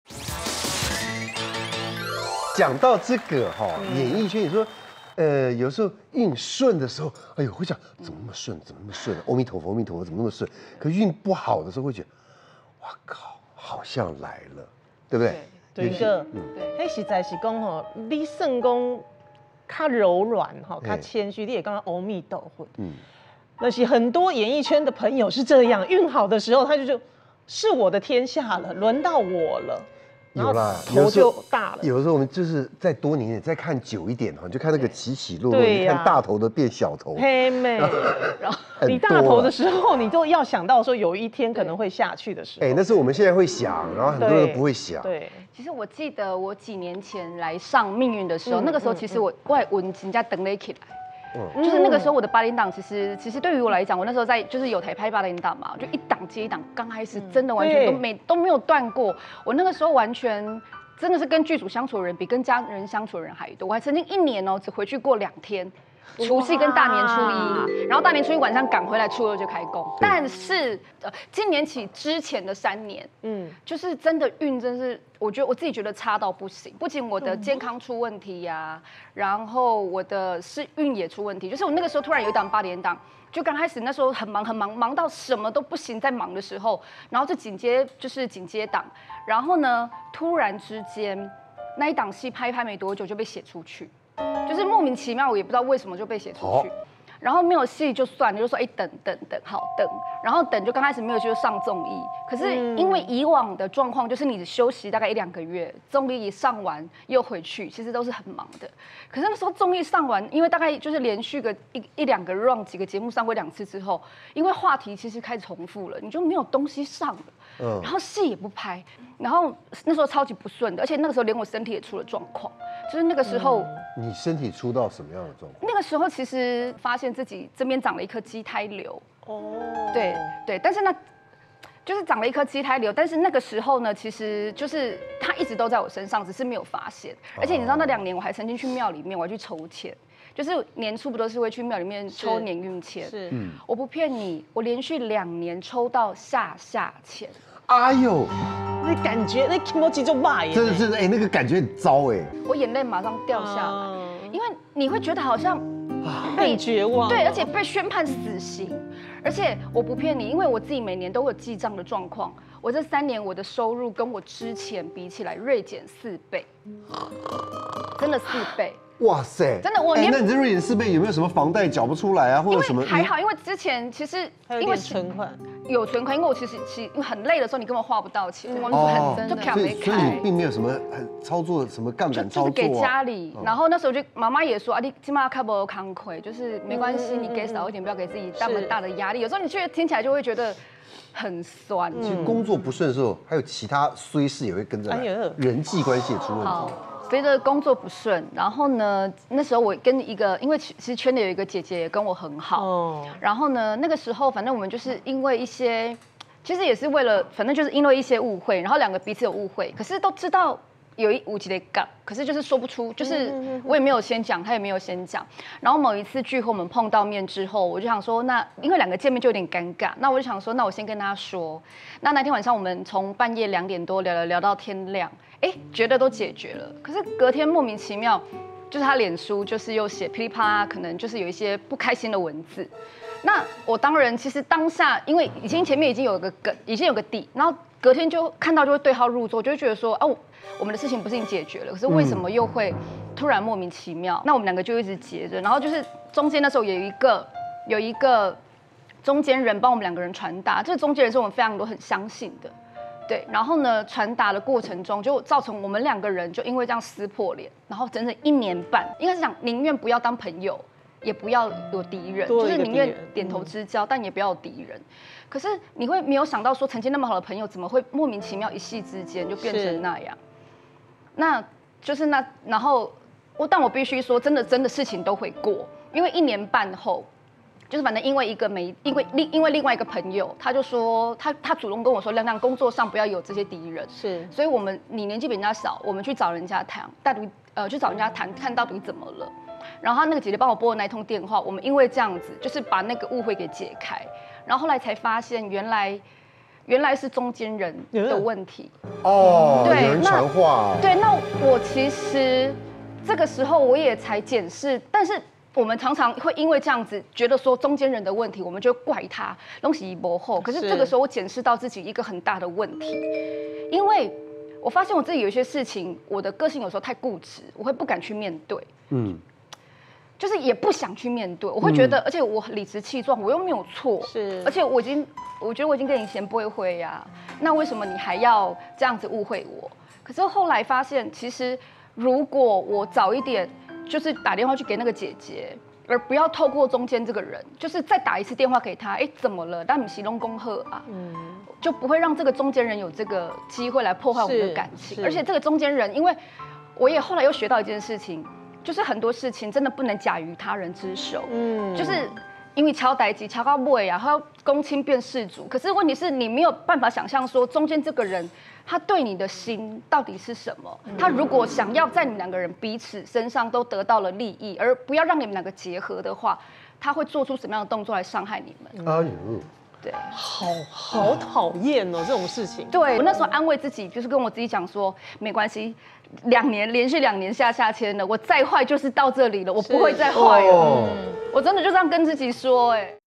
【精華版】李亮瑾罹畸胎瘤 拜神求籤瞬間落淚
在身體不適的時候工作也有一檔沒一檔，沒想到還求到下下籤。